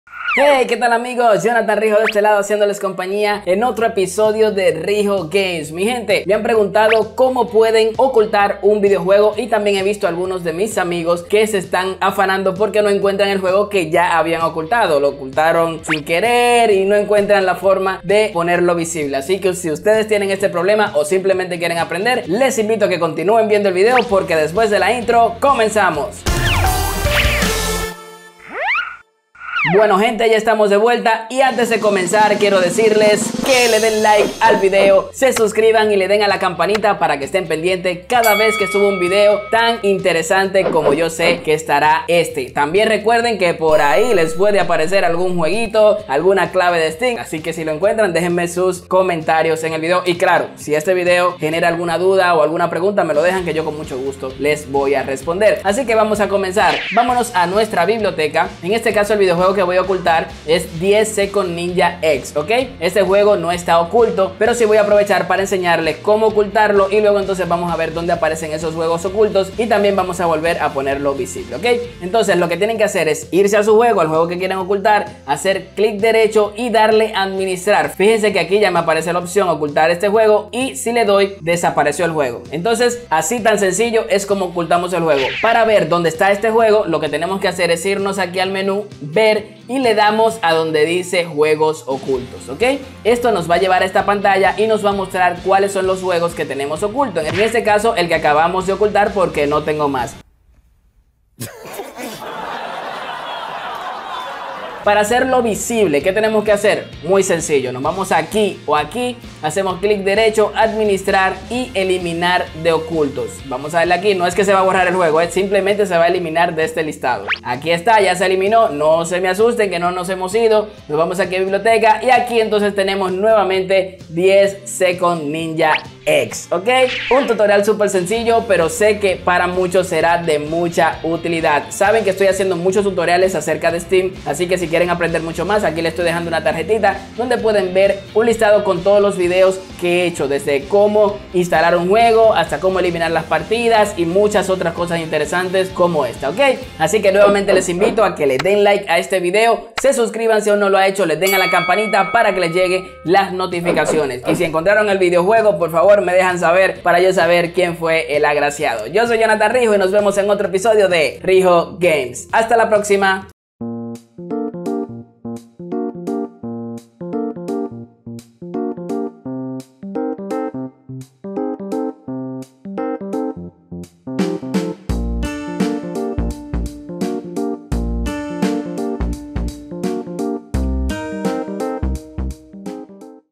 Como OCULTAR UN JUEGO en Steam 👍 Quitar un juego de oculto ❎
Aprende como a ocultar un juego en Steam en tu biblioteca, también como quitar un juego de oculto / desolcultar y a conocer donde se ocultan los videojuegos.

📑 MARCADORES
0:00 Comentario inicial (Importante)
0:26 Intro
0:29 INFORMACION IMPORTANTE
0:56 Como ocultar un juego steam
1:24 Donde están los juegos ocultos
1:44 Como hacer visible un juego oculto